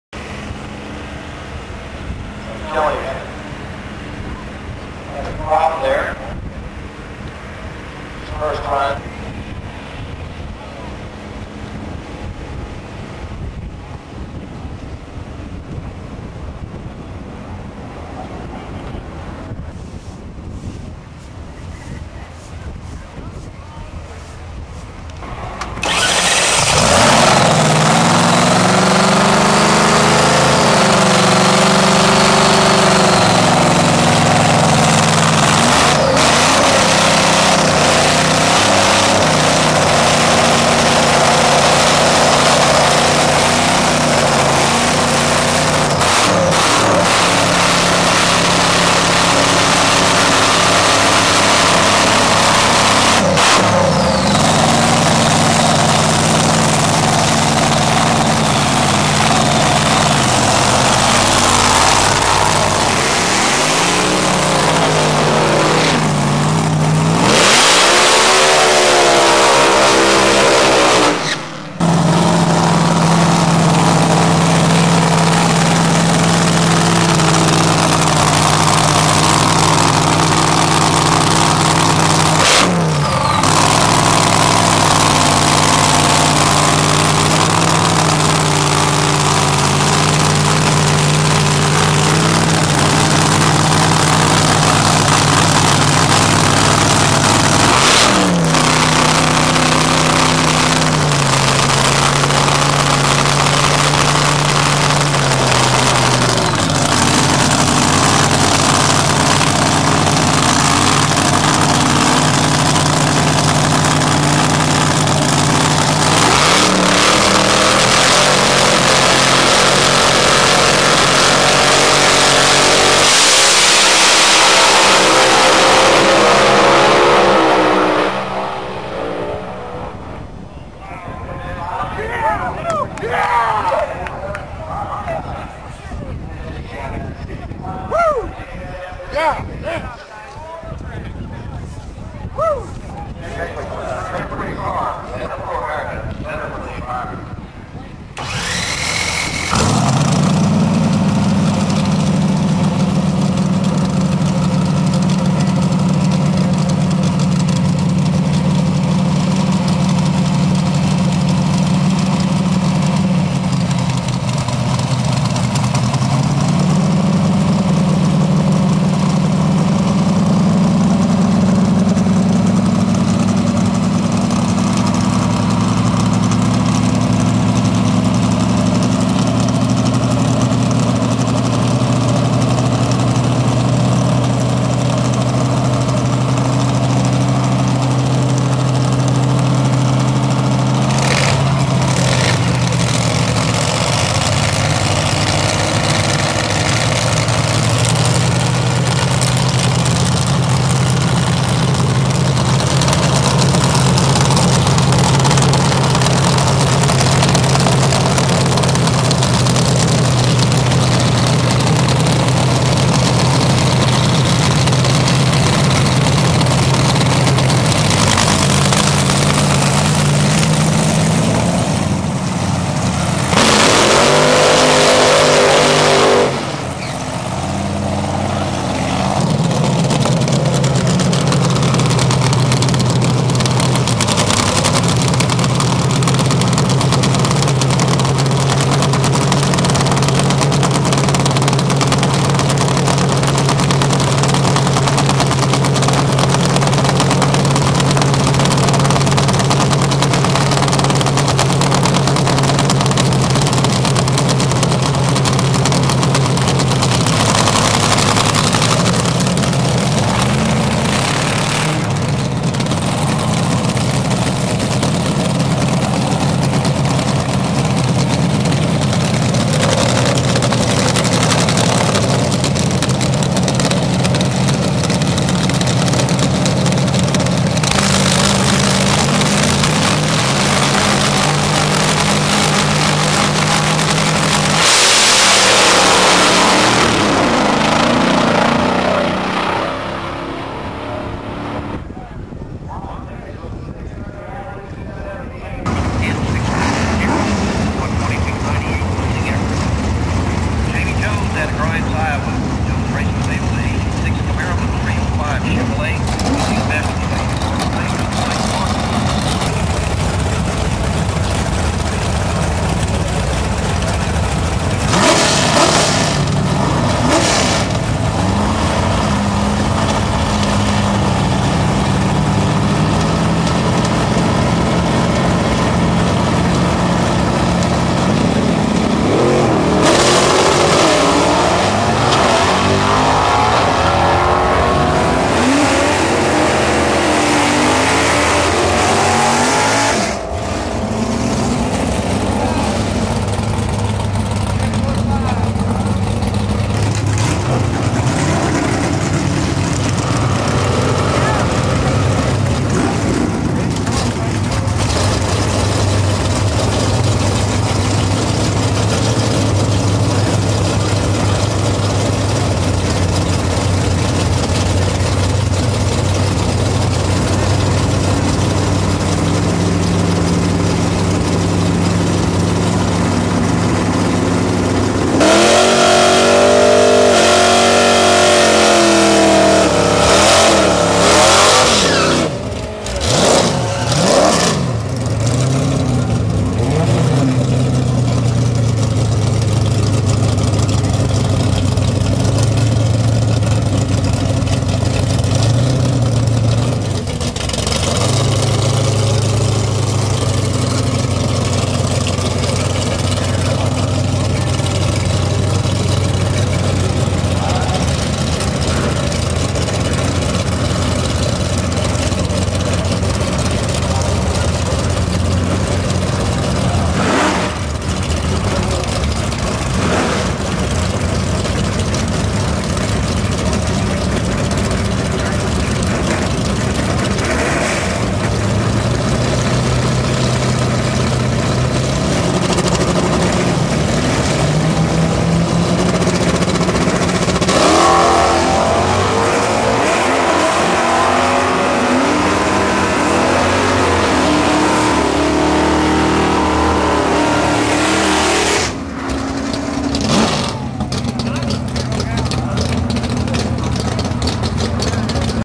2008 Div. 5 Divisional @ SRCA Dragstrip in Great Bend Part 1
Video from the 2008 NHRA Div. 5 divisional event at SRCA Dragstrip in Great Bend, Kansas